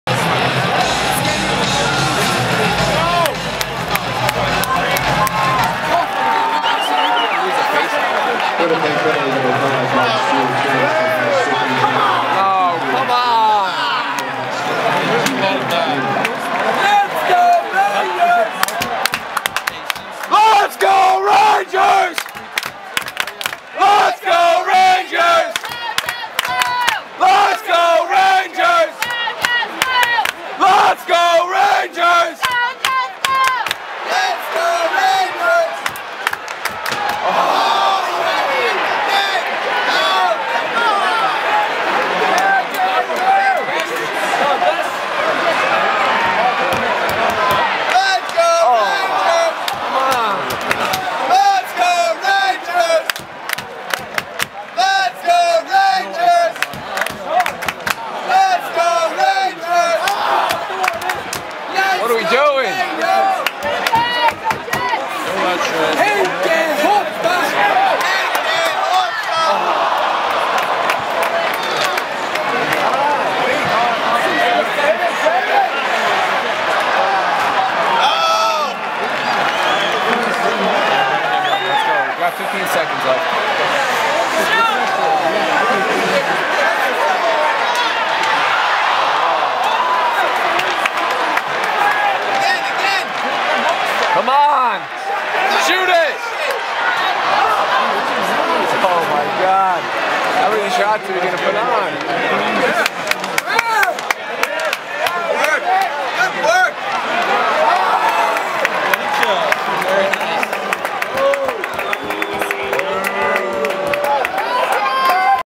New York Rangers vs. Winnipeg Jets HD
Some power play action, Jets players family were in front of us about 2 rows of them thats why theres some cheering for them haha